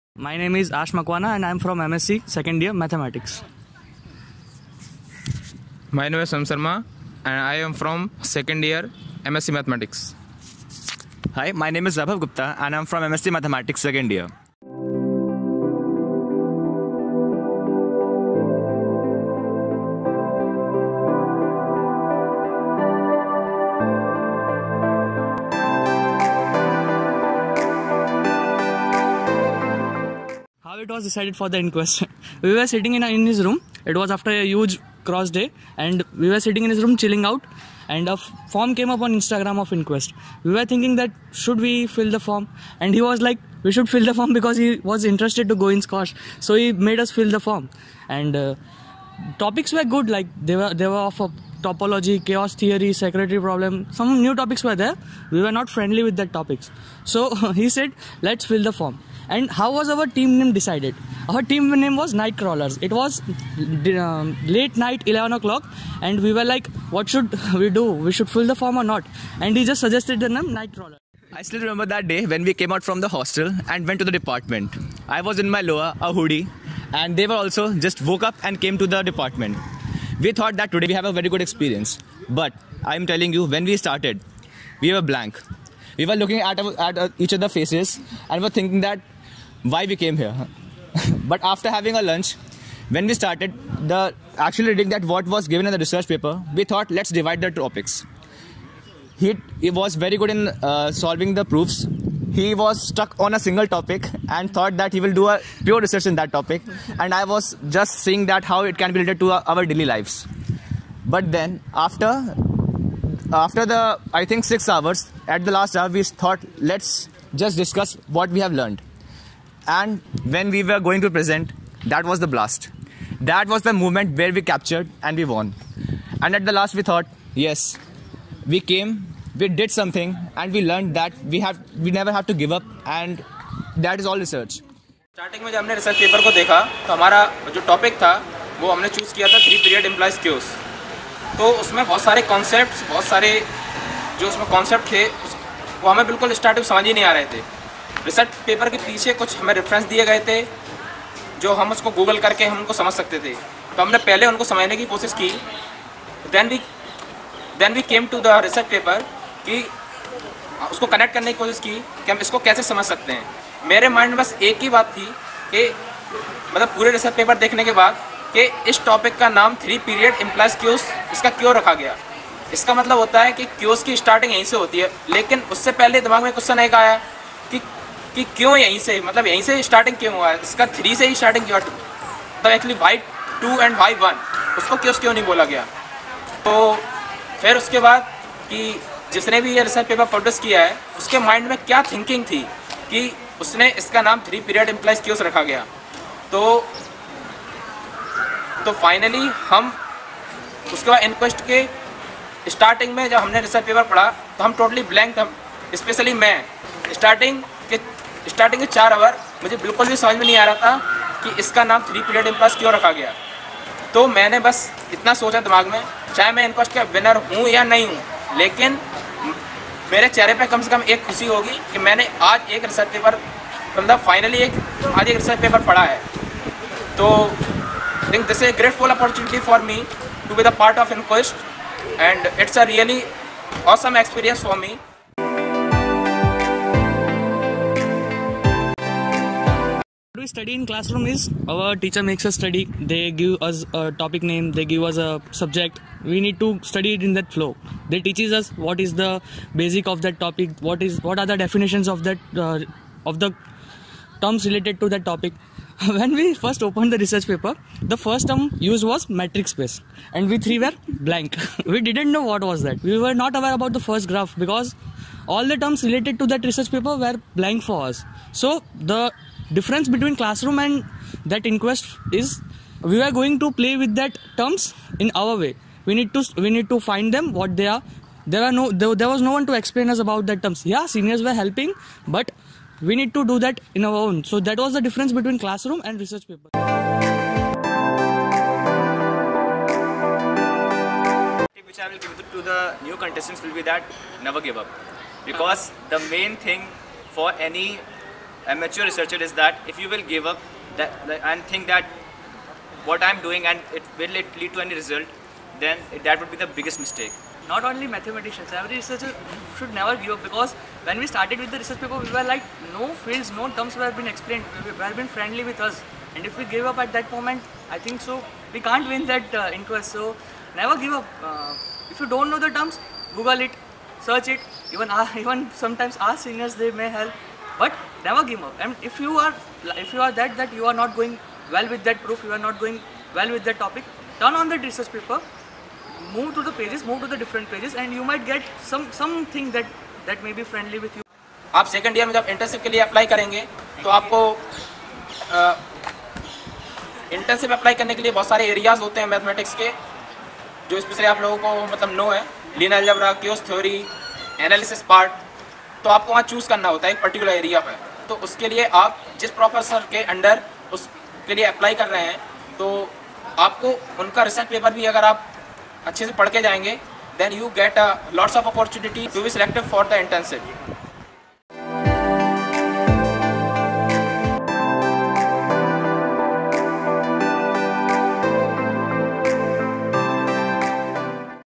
InQuest 3.0 Mathematics Winners | SCOSH | SVNIT | Researchathon
InQuest is aimed at popularizing research problems among UG students and give them an opportunity to have a tete-a-tete with a current research problem, so that it drives out the fear and doubt that comes to everyone when they first dive into the world of scientific literature.

This was the 3rd edition of the SVNIT's premiere Researchathon, which was successfully accomplishing its goal of popularizing reaserach problems among the scientific minds.